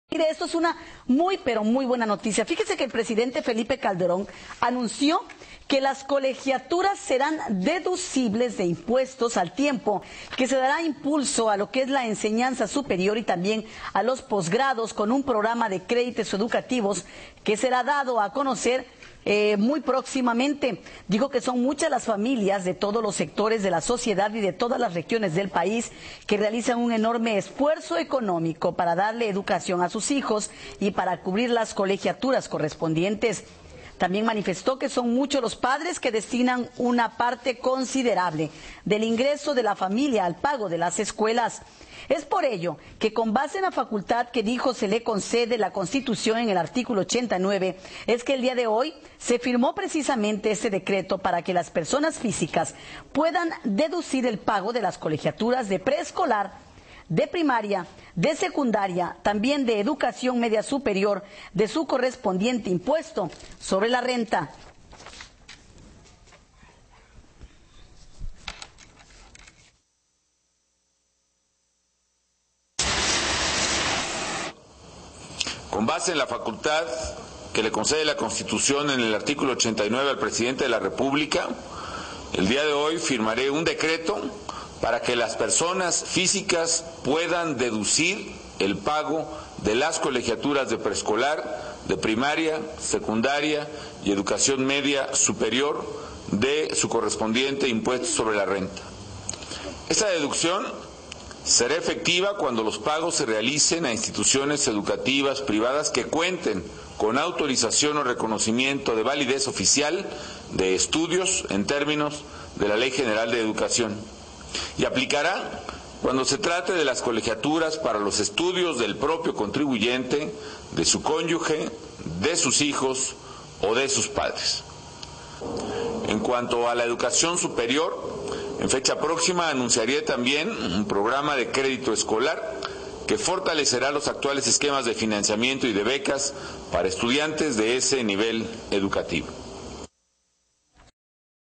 Deducibles de impuestos Colegiaturas
El Presidente Felipe Calderón anunció que firmará un decreto para hacer deducible el pago de colegiaturas de Preescolar, Primaria, Secundaria y Educación Media Superior.